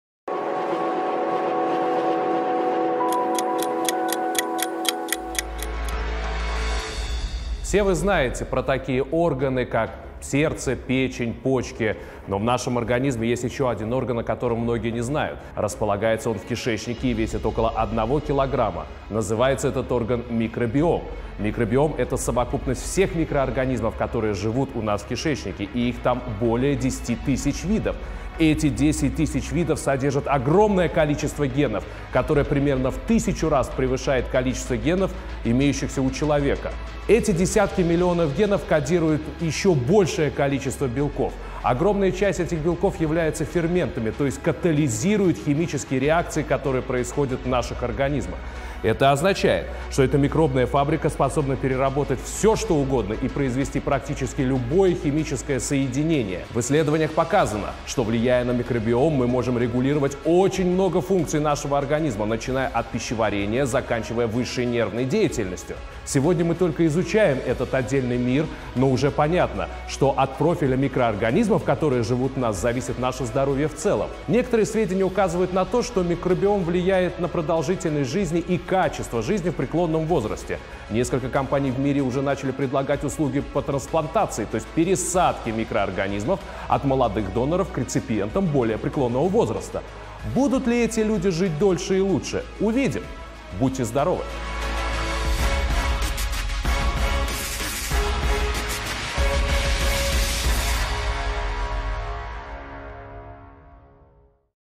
НАУКА ЗА МИНУТУ_Микробиом
В нашем организме есть орган, о котором многие не знают. Располагается он в кишечнике и весит около килограмма. Называется этот орган микробиом.

Видео создано в рамках проекта "НАУКА ЗА МИНУТУ" Медиацентром БФУ им. И. Канта.

Ведущий - кандидат биологических наук Максим Патрушев
Руководитель проекта - Дмитрий Бовин
Оператор - Максим Зацерклянный
Монтаж и компьютерная графика - Владимир Алексанин, Дмитрий Бовин